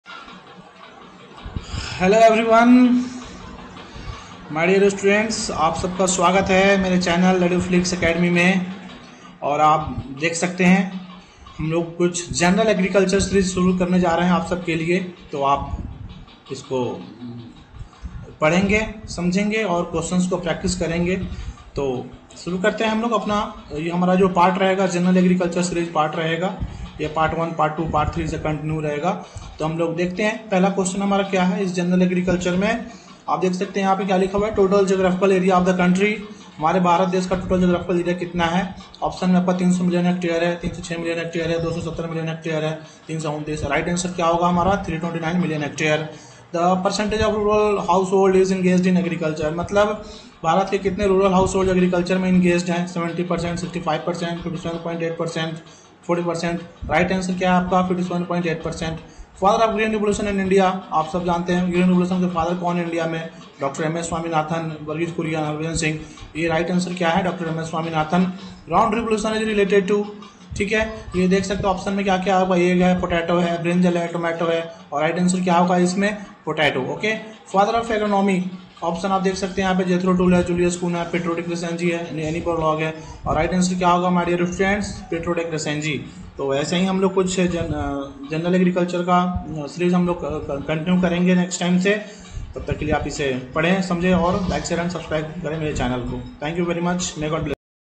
General agriculture shorts ytshorts icar nabard fci iffco nhb
This is all about general agriculture series question which is frequently asked in competitive examination.